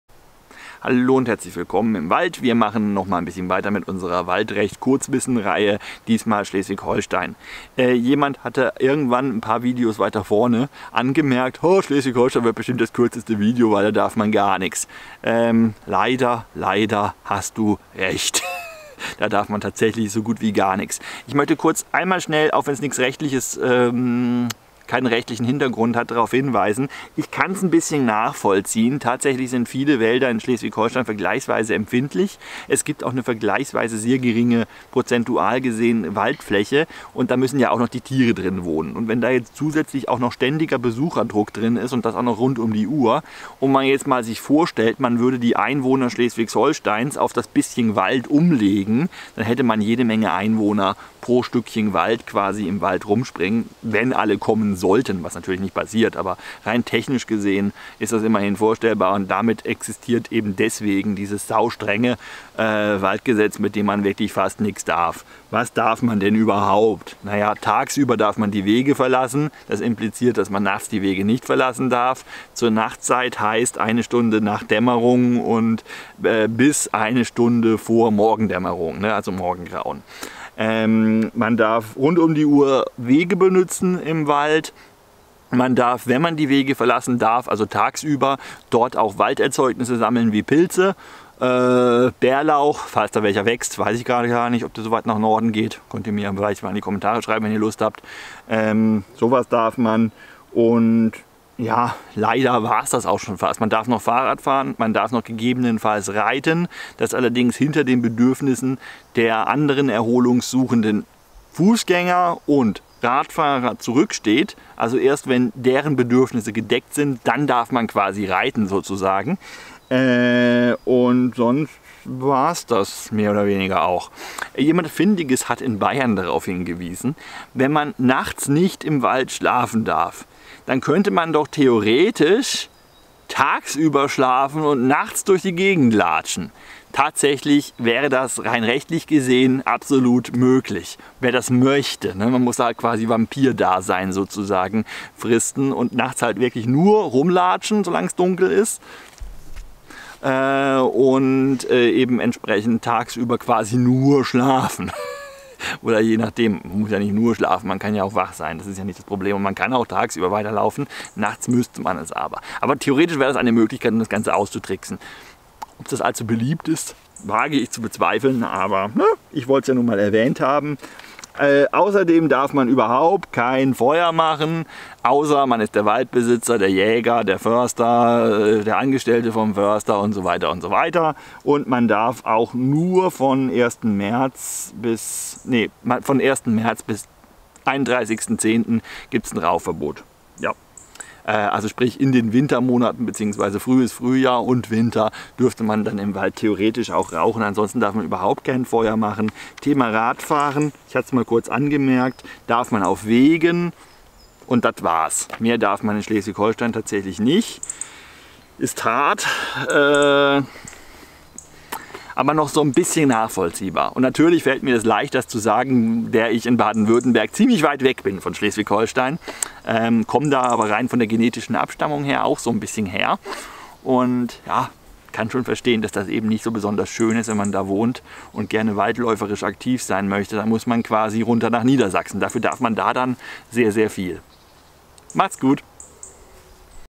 Schleswig Holstein - Waldbesuchsrecht nach Bundesländern (13)
Ihr dürft zwar nix. Dafür ist es sehr schön bei euch. Und ihr habt Inseln! Und Meer und Dänemark neben dran. Also ich würde ein paar Waldrechte gegen zumindest Nord ODER Ostsee eintauschen... ;-) Liefert sie einfach runter zum Schwarzwald! :-P